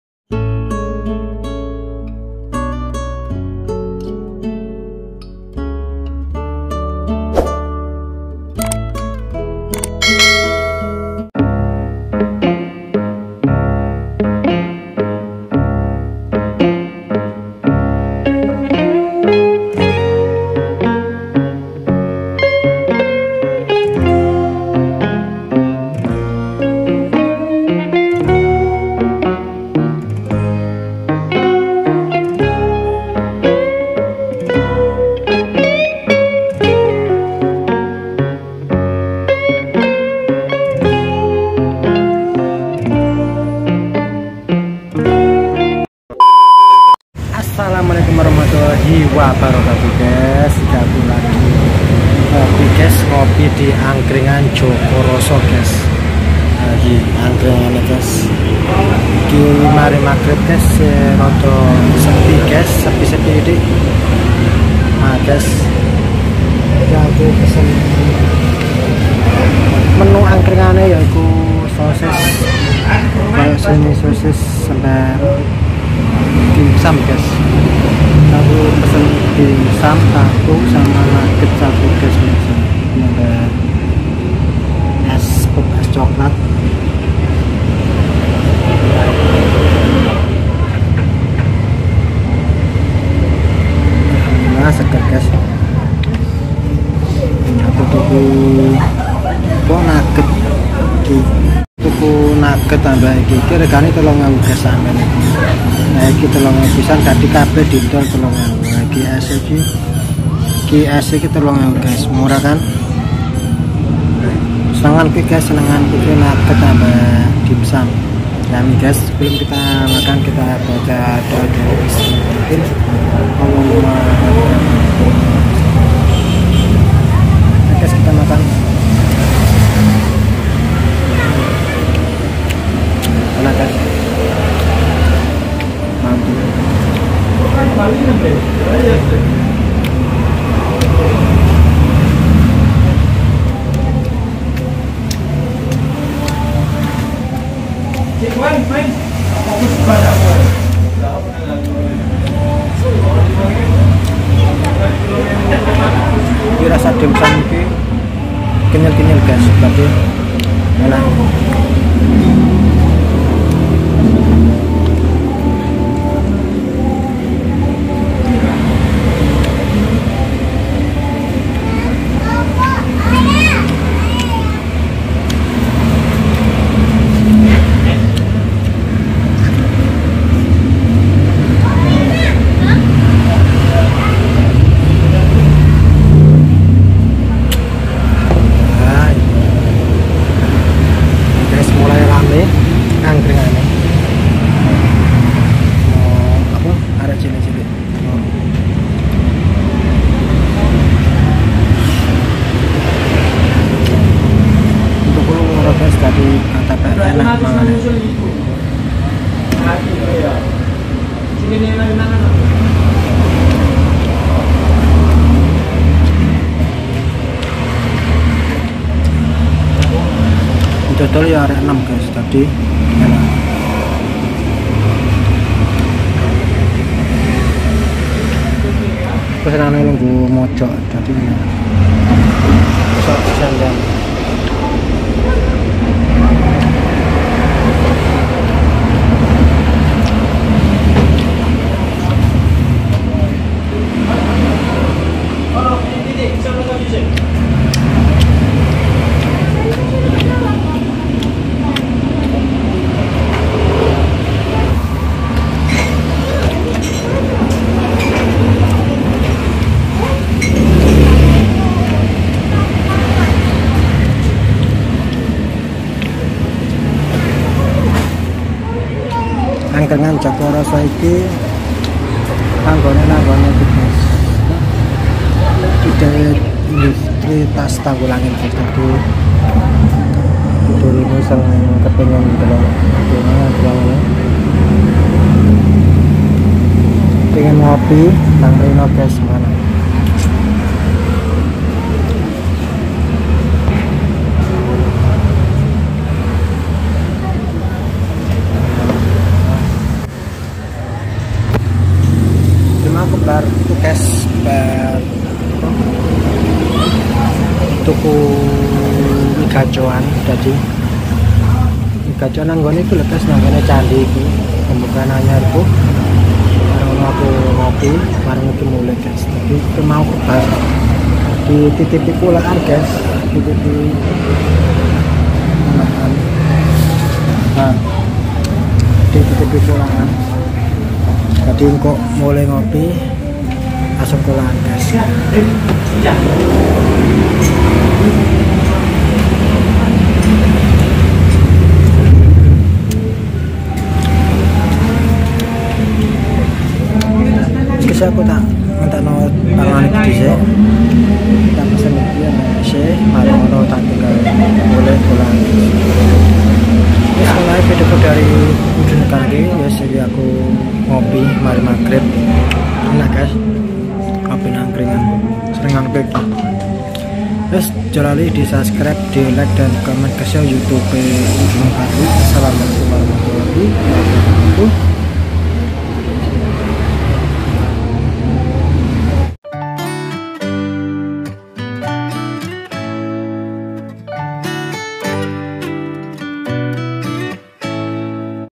MALAM INI KITA NGOPI DI ANGKRINGAN JOKO ROSO MENIKMATI SYAHDUNYA MALAM INI☕🥰😍😘
Hastag